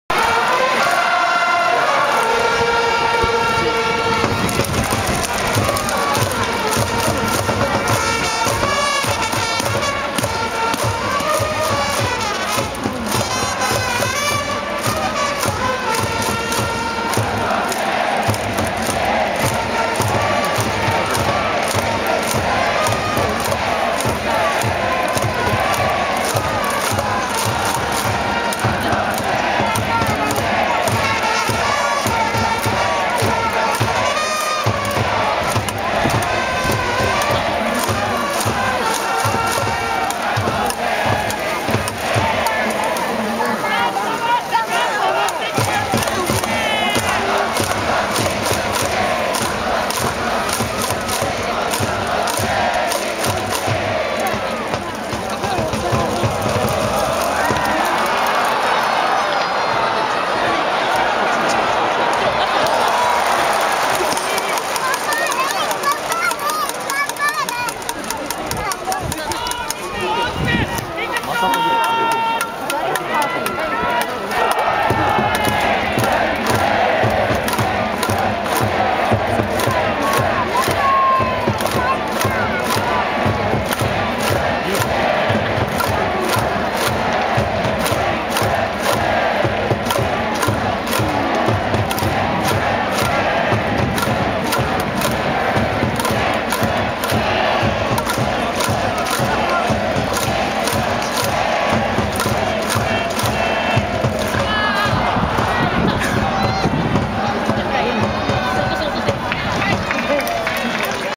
【ヒットエンドラン～！】広島カープ 菊池 涼介のバッティングフォーム_2024030202
2024年オープン戦、東北楽天ゴールデンイーグルス VS 広島東洋カープ@倉敷マスカットスタジアムでの広島カープ 菊池 涼介のバッティングフォームです。(遠くからの撮影でブレが大きめです。すみません。)